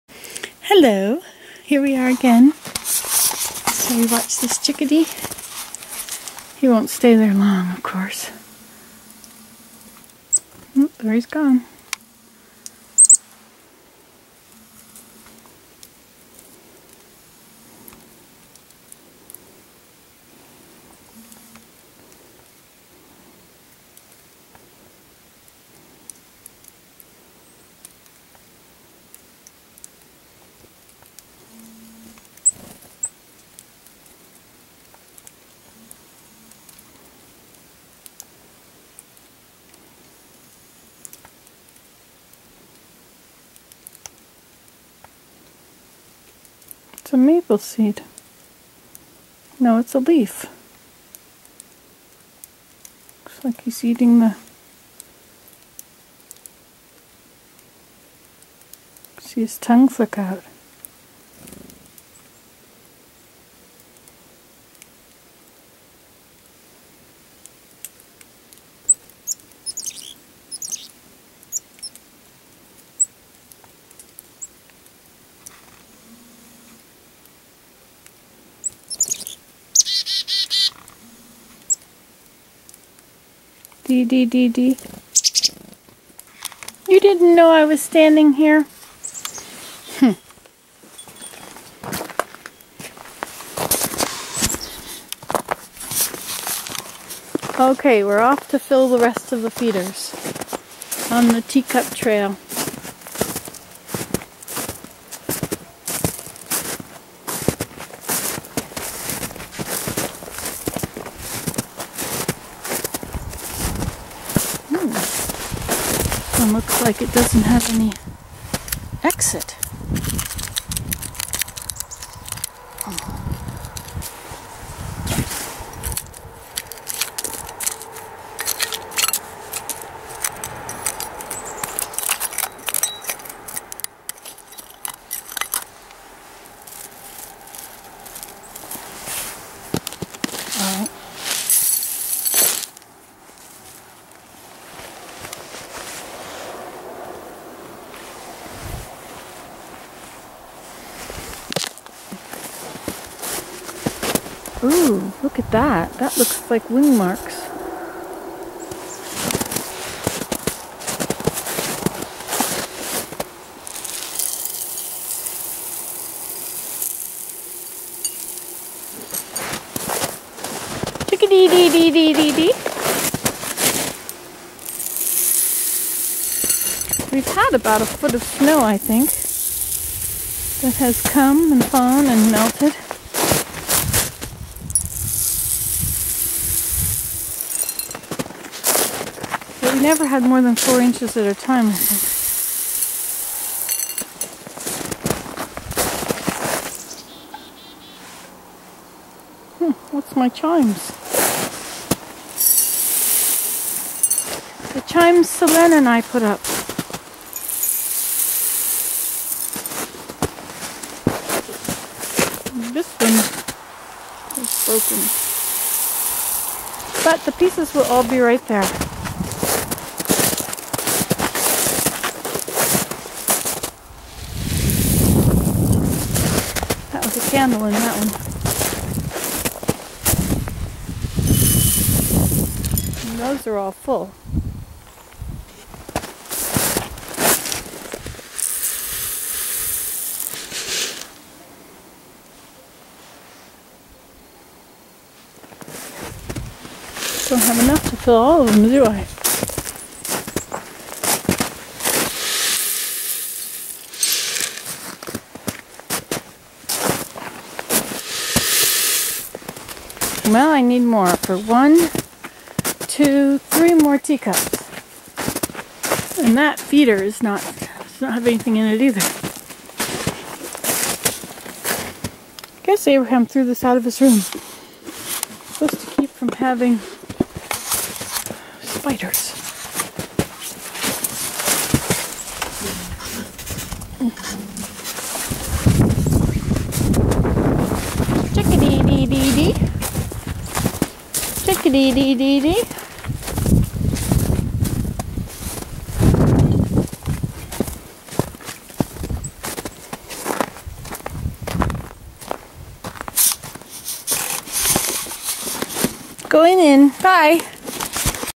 Nancy Today: Teacup Trail chickadee and icecubes ASMR Birds 'Sounds of Nature'
Wandering through the Teacup Trail with birdseed